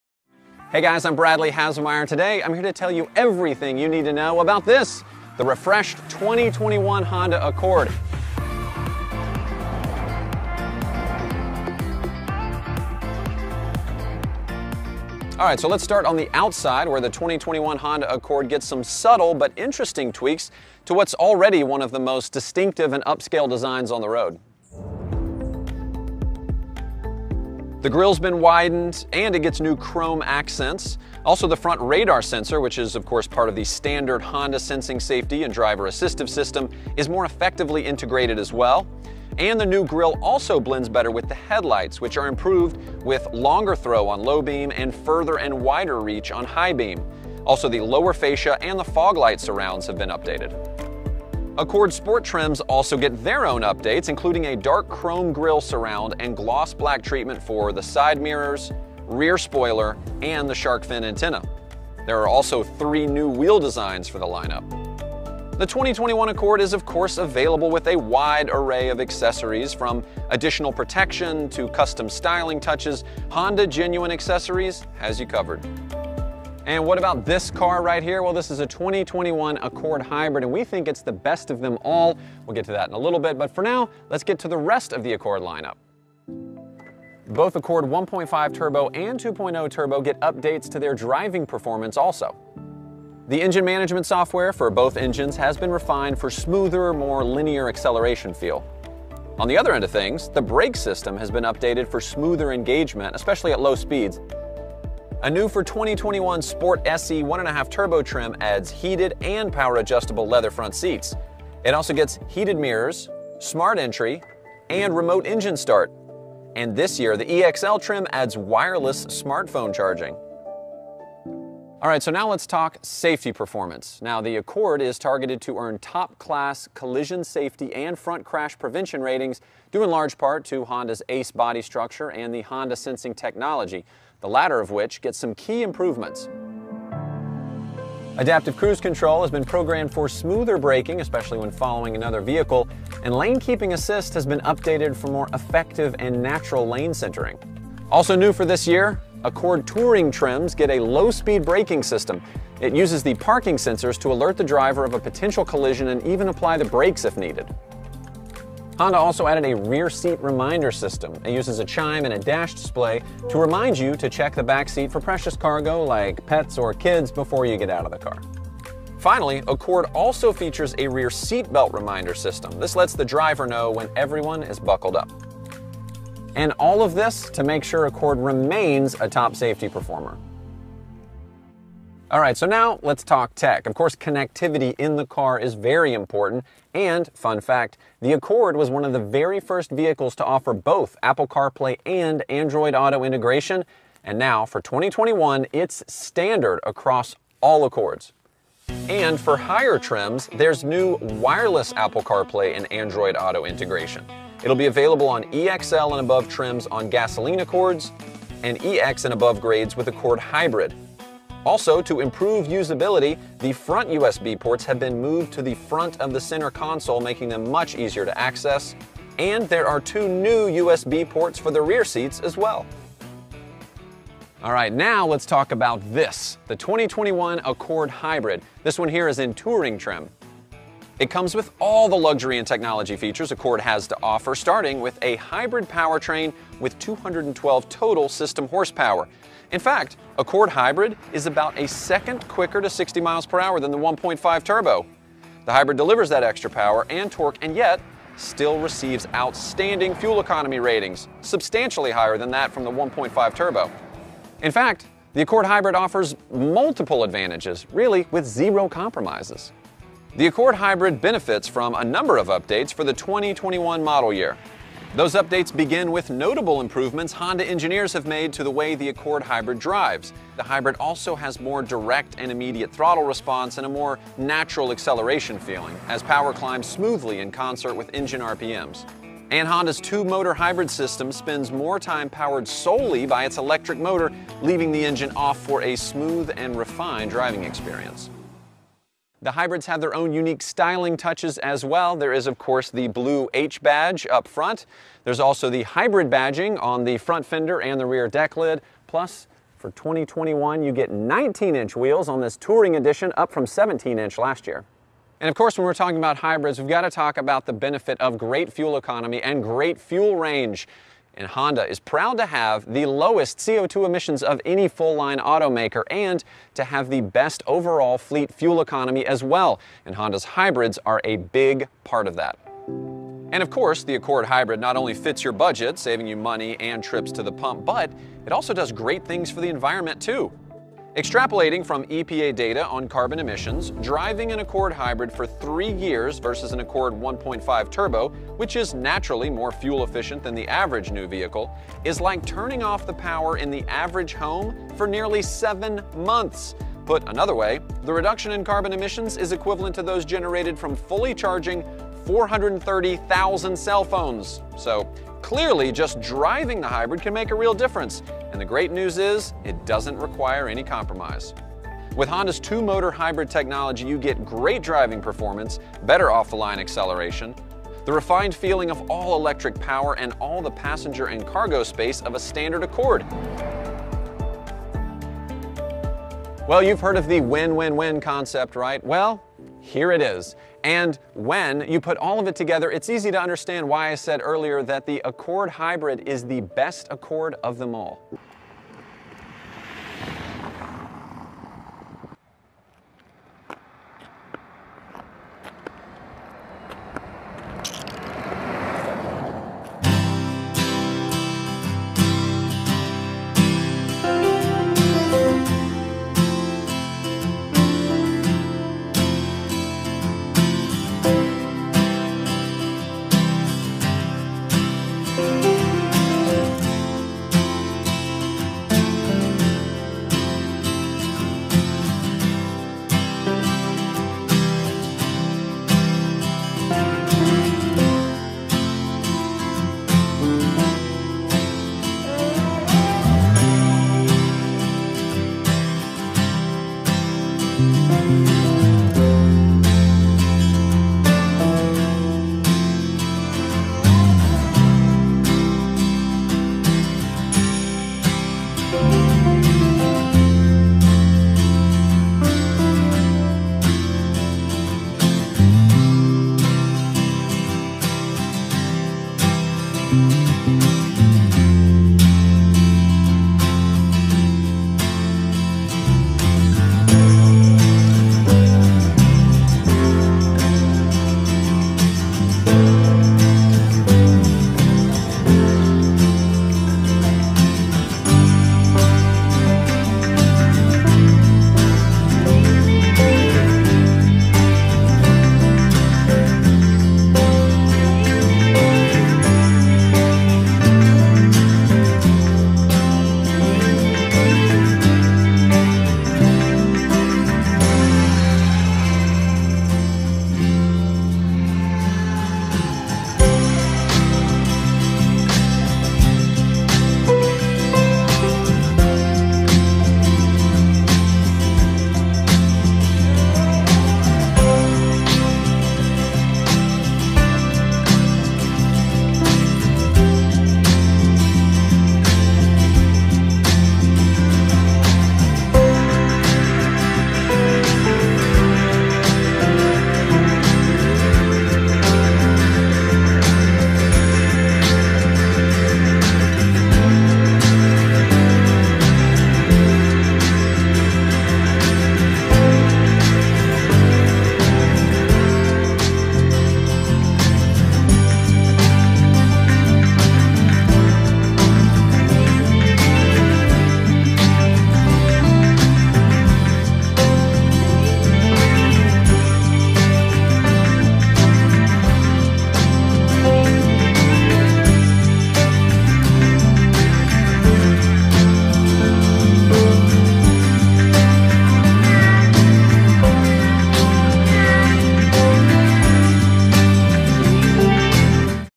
2021 New Honda Accord facelift - Full Review - Rival Toyota Camry and Mazda 6. Price at $25,725
2021 New Honda Accord facelift - Full Review Interior, Exterior - Rival Toyota Camry and Mazda 6.
2021 Honda Accord Gets Refreshed Styling, Updated Hybrid Variant, New Sport Special Edition Trim, plus Wireless Apple CarPlay® and Android Auto™ Integration
Widely regarded by experts as the benchmark midsize sedan, Honda raises the Accord's game for the 2021 model year with a refresh that includes updates to styling, technology, drivability and safety features. In addition, unique upgrades were made to performance and design for the ultra-efficient and refined Accord Hybrid. On-sale at Honda dealerships beginning Oct. 13, the 2021 Accord will carry a Manufacturer's Suggested Retail Price (MSRP) starting at $24,770 (not including destination and handling), while the 2021 Accord Hybrid will be available at a starting MSRP of $26,370.
VTEC Turbo® 1.5-liter, DOHC direct-injected inline-4 engine with 192 HP/192 lb.-ft. torque
VTEC Turbo® 2.0-liter, DOHC direct-injected inline-4 engine with 252 HP/273 lb.-ft. torque
Honda Accord 2021
Honda Accord 2020
Honda Accord facelift
Honda Accord interior 2021